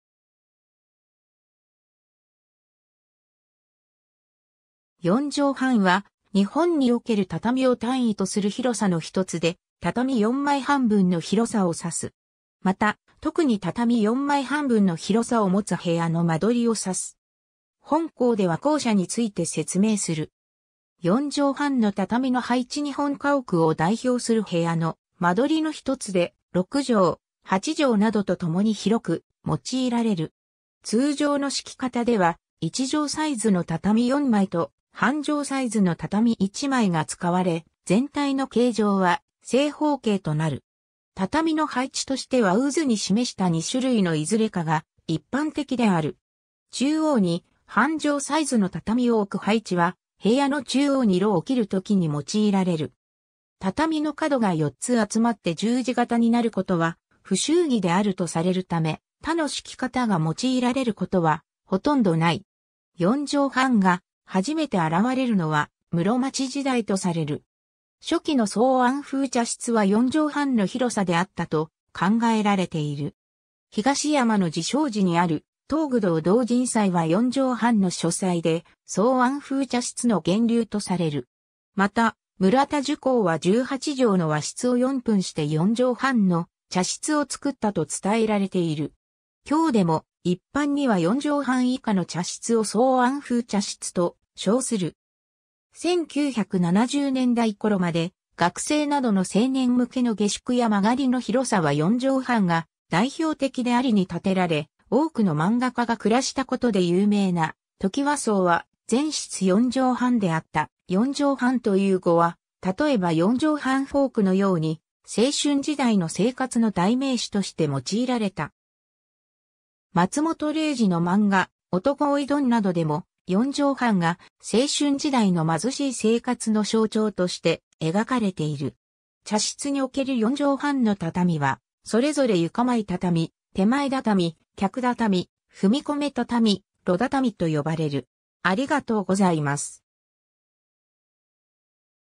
四畳半
四畳半（よじょうはん）は、日本における畳を単位とする広さのひとつで、畳4枚半分の広さを指す。
また、特に畳4枚半分の広さを持つ部屋の間取りを指す。
本項では後者について説明する。
 四畳半の畳の配置 日本家屋を代表する部屋の間取りのひとつで、六畳、八畳などとともに広く用いられる。
通常の敷き方では、一畳サイズの畳4枚と半畳サイズの畳1枚が使われ、全体の形状は正方形となる。
畳の配置としては右図に示した2種類（及びその鏡面形）のいずれかが一般的である。
中央に半畳サイズの畳を置く配置（卍敷きとも呼ばれる）は、部屋の中央に炉を切る時に用いられる。
畳の角が4つ集まって十字形になることは不祝儀であるとされるため、他の敷き方が用いられることはほとんどない。
 四畳半が初めて現れるのは室町時代とされる。
 初期の草庵風茶室は四畳半の広さであったと考えられている。
東山の慈照寺（銀閣寺）にある東求堂同仁斎（1486年（文明18年））は四畳半の書斎で、草庵風茶室の源流とされる。
また、村田珠光は18畳の和室を4分して四畳半の茶室を造ったと伝えられている。
今日でも、一般には四畳半以下の茶室を草庵風茶室と称する。
 1970年代頃まで、学生などの青年向けの下宿や間借りの広さは四畳半が代表的であり（例えば、1952年（昭和27年）に建てられ、多くの漫画家が暮らしたことで有名なトキワ荘は全室四畳半であった）、「四畳半」という語は、例えば四畳半フォークのように、（当時の）青春時代の生活の代名詞として用いられた。
松本零士の漫画『男おいどん』などでも「四畳半」が青春時代の貧しい生活の象徴として描かれている。
 茶室における四畳半の畳は、それぞれ床前畳（貴人畳）、点前畳（道具畳）、客畳、踏込畳、炉畳と呼ばれる。